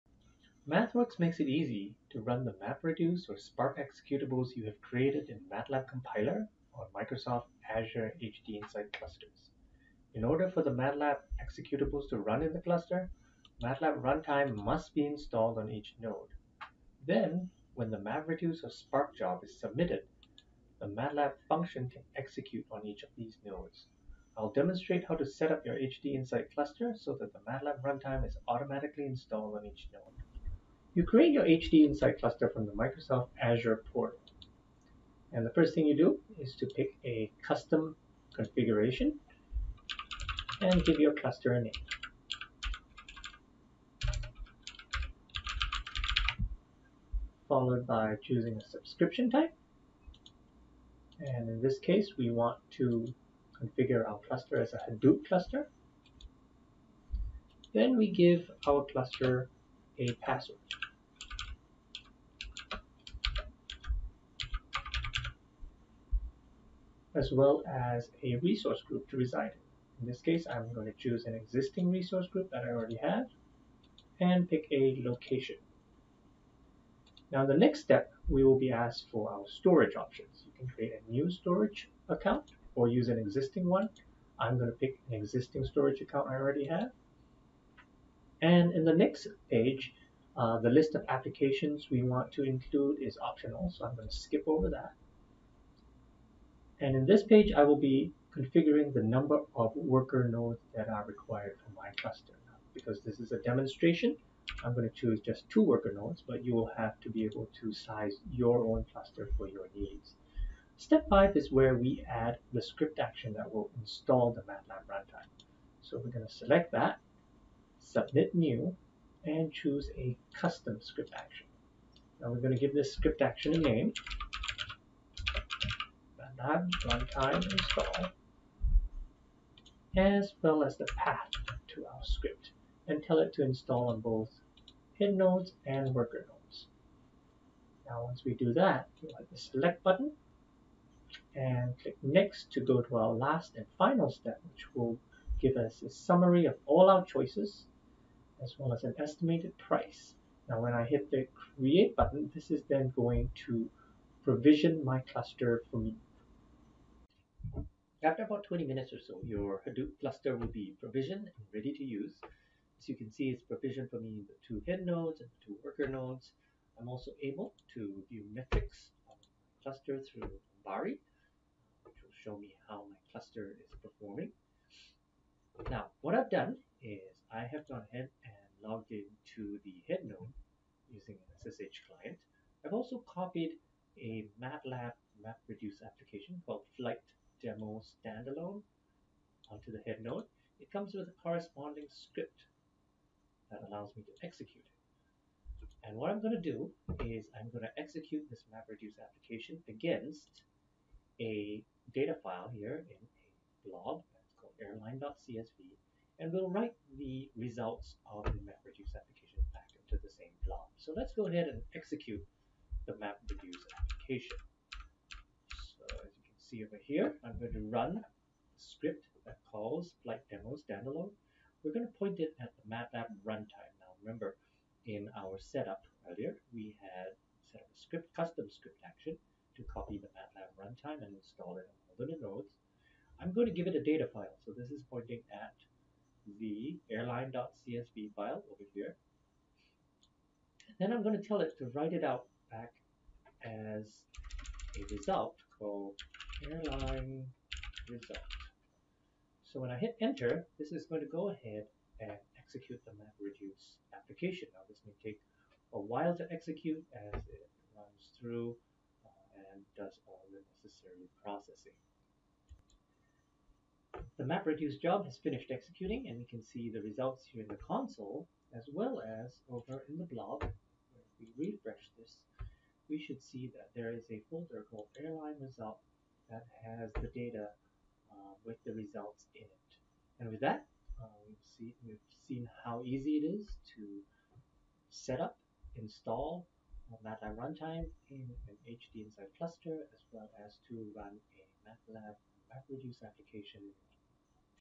Getting Started: Deploying MATLAB Big Data Applications to Microsoft HDInsight
MATLAB Compiler™ allows you to create map-reduce or Spark™ applications that can run in Azure® HDInsight, a managed analytics service running on the Microsoft® cloud platform. MATLAB Runtime must be installed on every node in the cluster, and MathWorks provides an automated script to streamline the process.